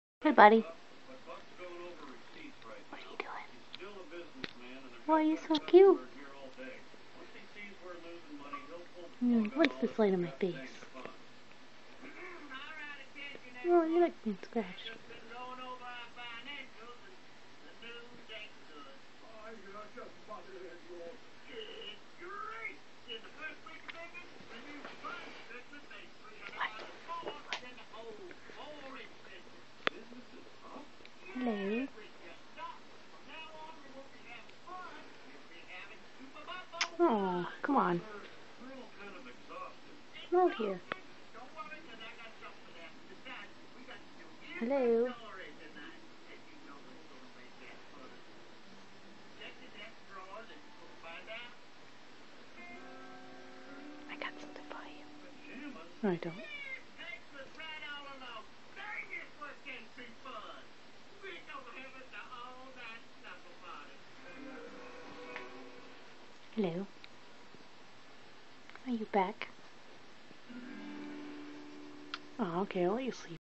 Pocky the Sleepy Flying Squirrel
My flying squirrel, Pocky, now almost exactly a year old. First video of him was sort of popular, so I thought I'd upload this one.

I think he was afraid of the camera.. he usually hangs off the side of the bed like that and then plops down in my hand after I scratch behind his ears. I never pick him straight up out of his hammock because it scares him, so I let him come to me.